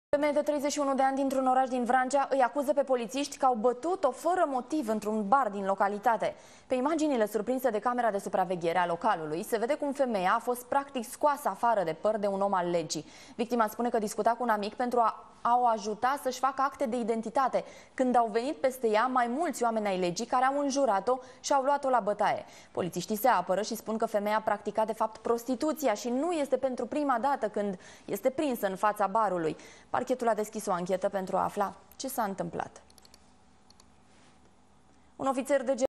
Femeie batuta de politisti in Vrancea 15 IANUARIE 2012
O femeie de 31 de ani dintr-un oras din Vrancea ii acuza pe politisti ca au batut-o fara motiv intr-un bar din localitate. Pe imaginile surprinse de camera de supraveghere a localului se vede cum femeia a fost practic scoasa afara de par de un om al...